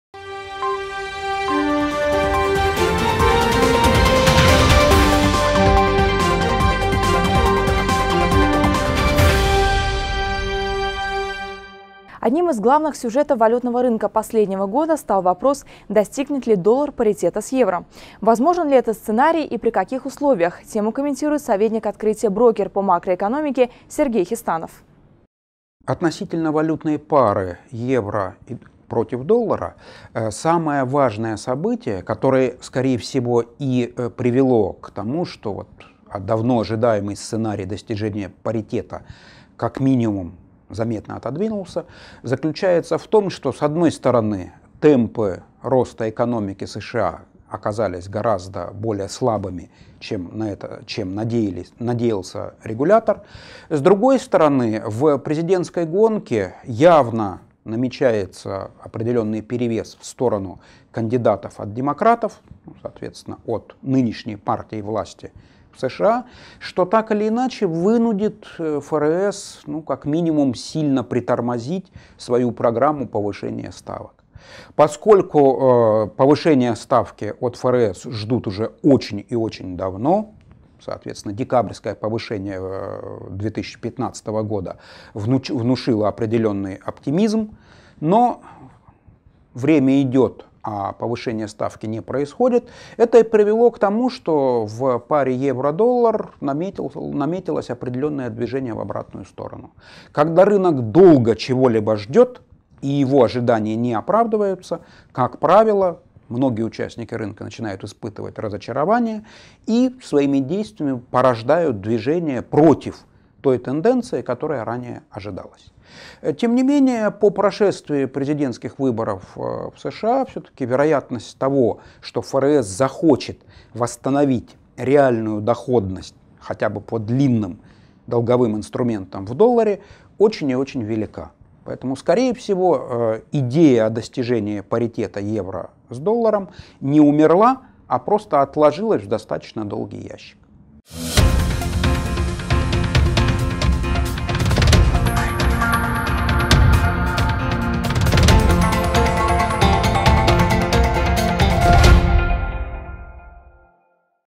Сергей Хестанов - "Паритет доллара с евро" | 27.07.2016
Одним из главных сюжетов валютного рынка последнего года стал вопрос, достигнет ли доллар паритета с евро. Возможен ли этот сценарий и при каких условиях тему комментирует советник “Открытие Брокер” по макроэкономике Сергей Хестанов.